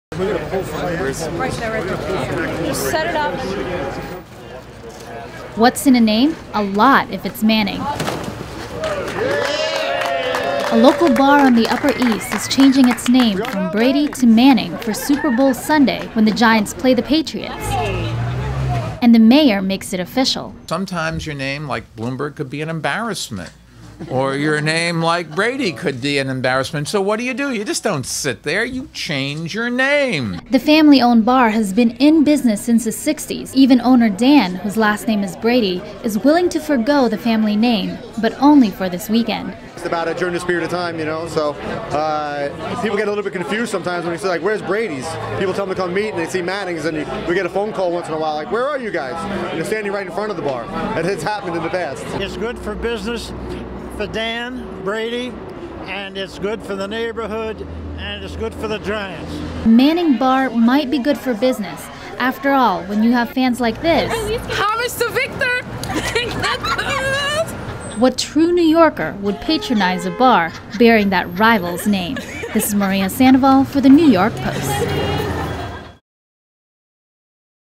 UES sports bar gets name chang - New York Post
'Brady's, a popular bar on the upper east side temporarily changes its name to 'Manning's' for Super Bowl Sunday.